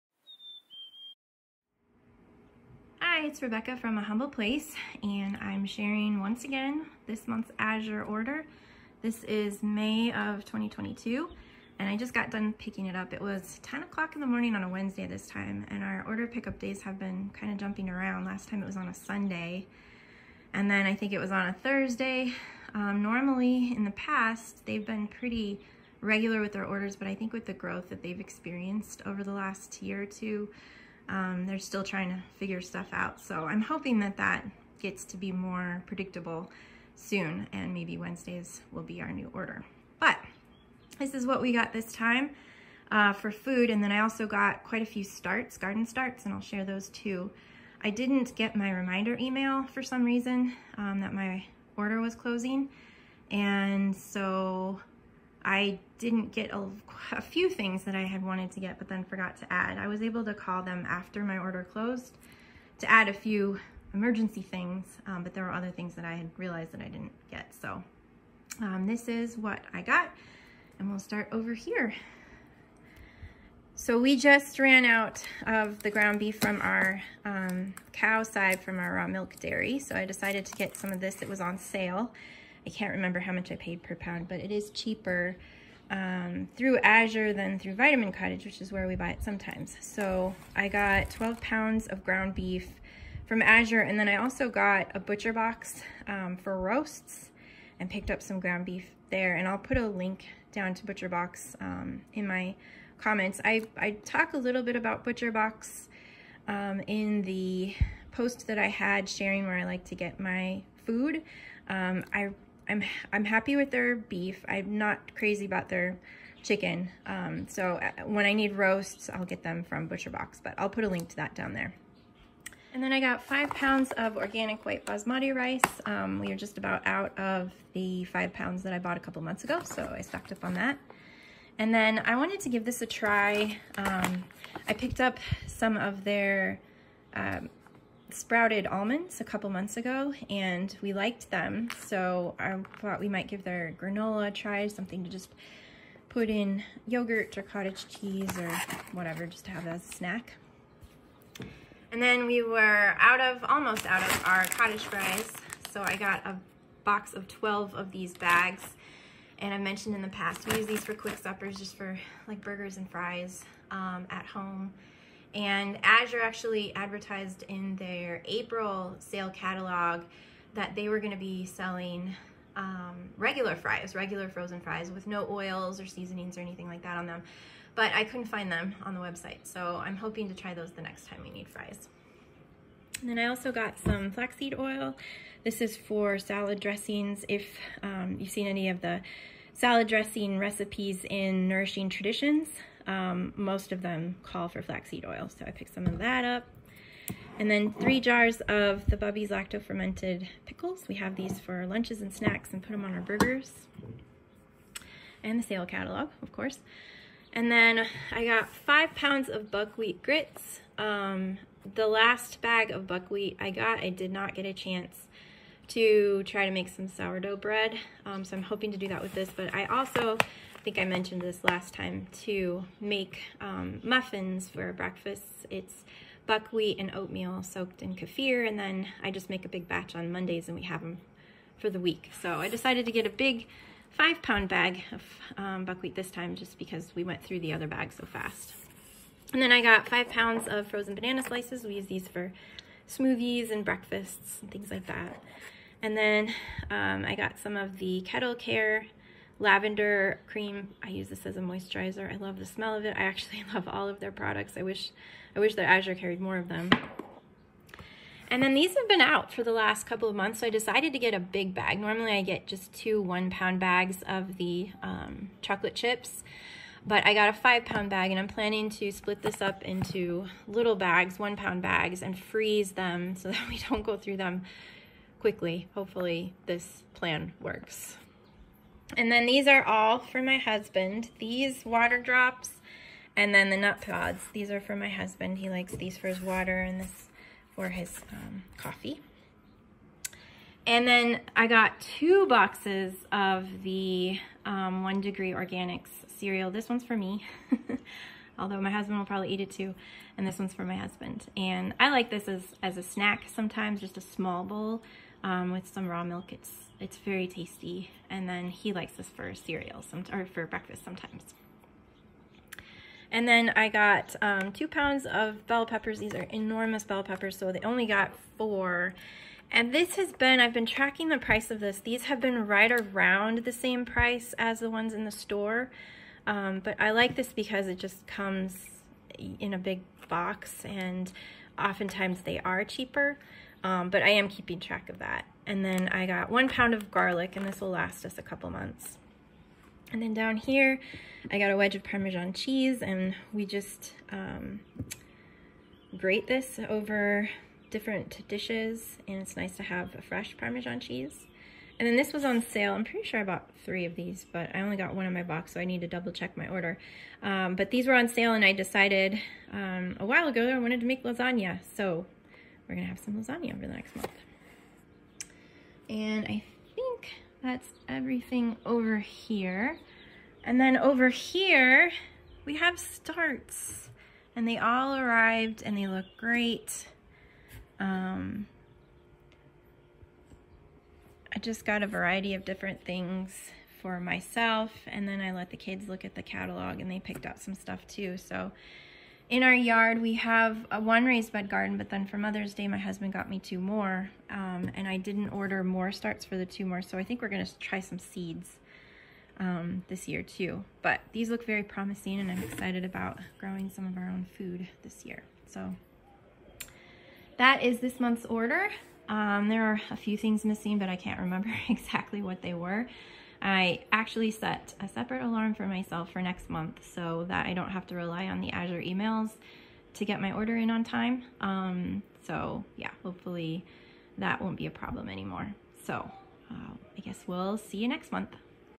May 2022 Azure Haul (with prices!)
Quick video this month as our pick-up was earlier in the day and I had other things to get to. Here's what we got this time!

* 6 x 10 oz One Degree organic sprouted brown rice cacao crisps cereal
* 2 lb organic red bell peppers
* 12 lb Thousand Hills organic grass-fed ground beef
* 5 lb Lundberg organic white basmati rice
* 9 x 1.62 floz Sweet Leaf Natural Water Drops
* 12 x 16 oz SnoPac frozen organic cottage fries
* 12 x 11.2 floz Nutpods French vanilla creamer
* 2 oz Kettle Care Balancing Lavender Cream
* 16 floz Azure Market organic flaxseed oil
* 30 floz Coconut Secret coconut aminos (I didn't talk about this in the video - we use it in place of soy sauce)
* 6 x 8 oz One Degree organic sprouted oat o's cereal
* 5 lb Azure Market organic, allergen-free, fair trade chocolate chips
* 8 oz Lark Ellen Farm grain-free, sprouted vanilla cinnamon granola
* 5 lb Azure Market organic broken buckwheat groat grits
* 13 oz Jovial brown rice lasagna sheets
* 3 x 33 oz Bubbies lacto-fermented pickles
* 5 lb organic frozen banana slices
* 8 oz Rumiano organic parmesan cheese wedge
* 1 lb organic garlic
* various garden starts!

Links:
Butcher Box (get $30 off your first order!)

List of all of the things I currently buy from Azure (as well as how it works)